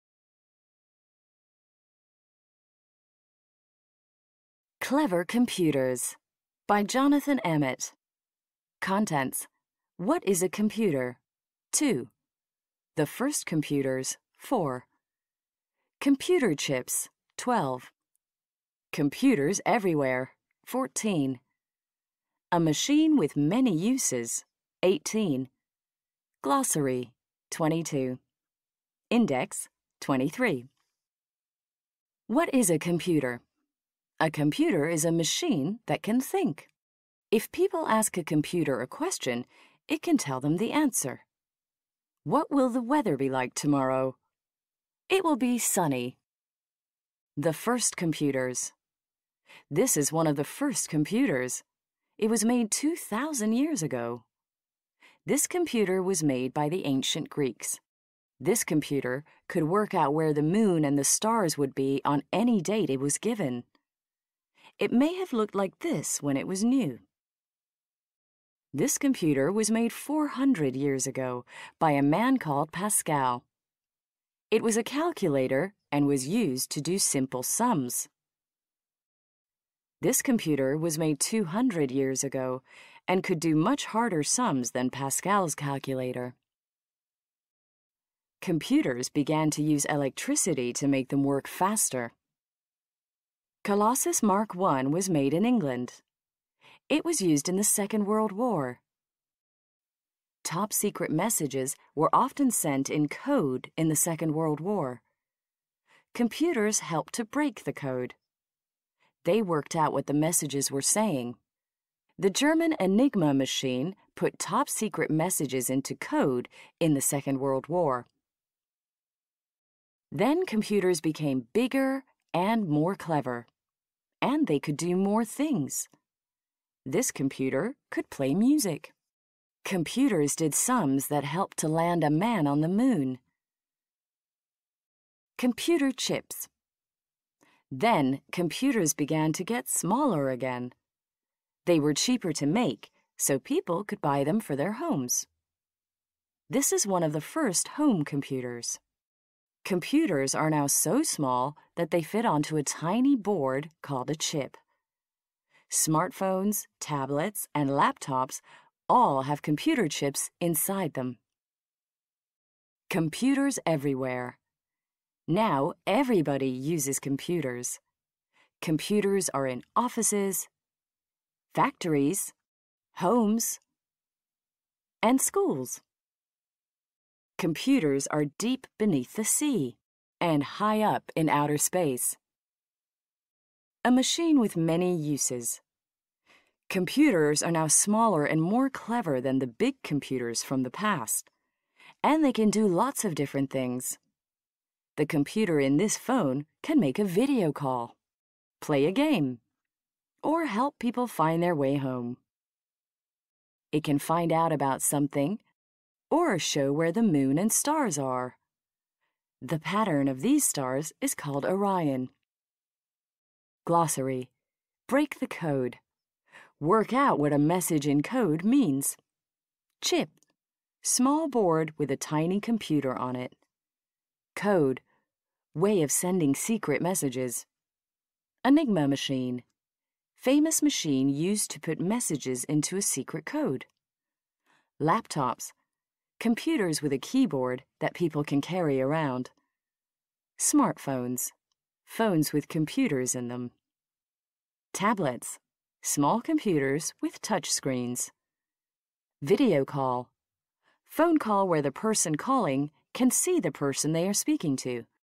Clever Computers - American English - Cambridge Reading Adventures Turquoise
Cambridge Reading Adventures Turquoise
Clever Computers - American English.
Computers are used in every part of life. Discover how they developed to the machines of today.